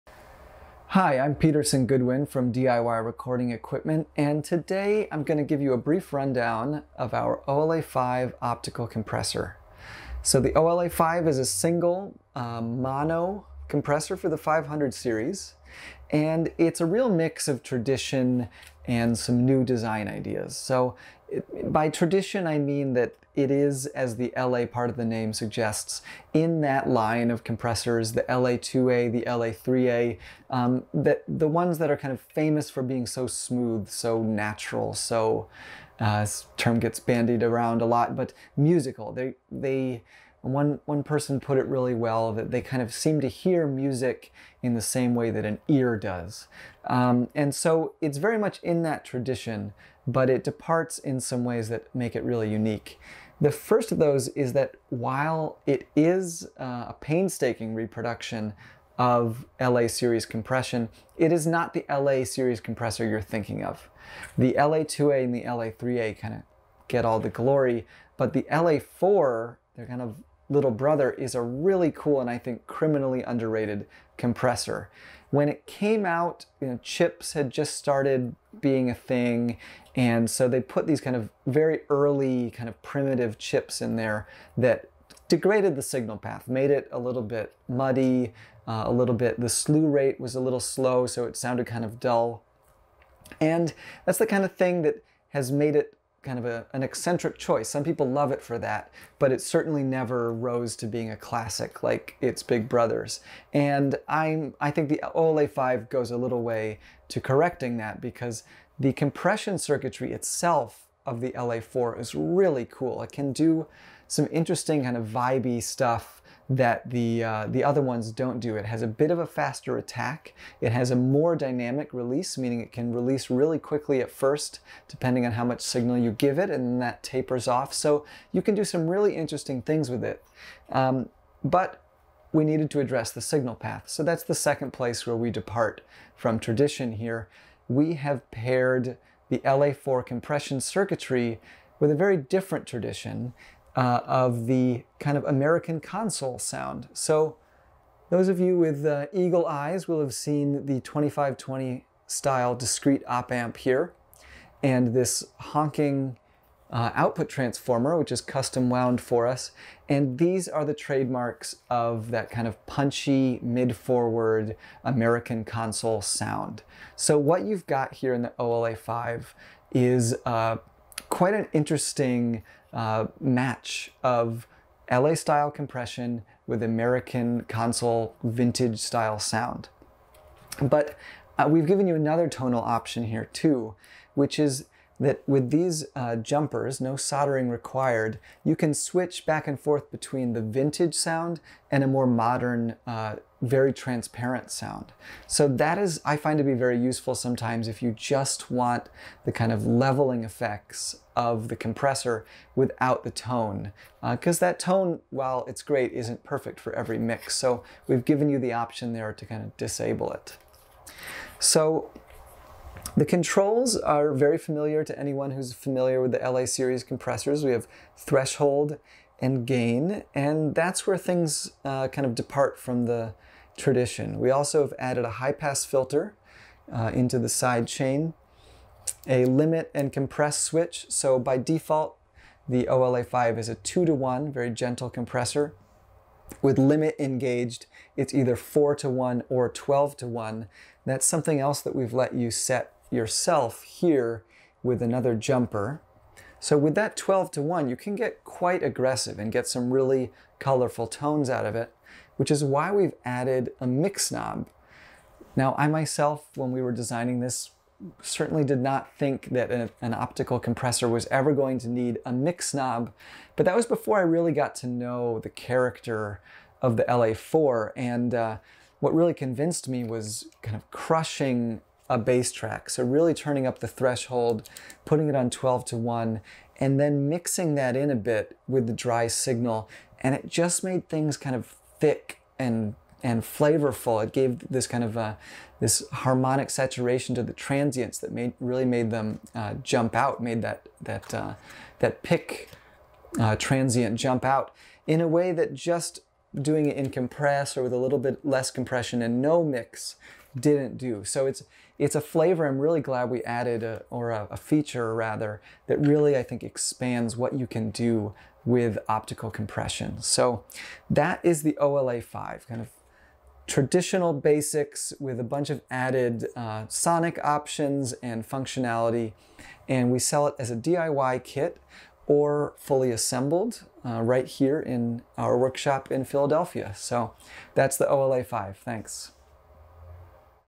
OLA5 500-Series Optical Compressor Overview
A quick rundown of our OLA5 Optical Compressor.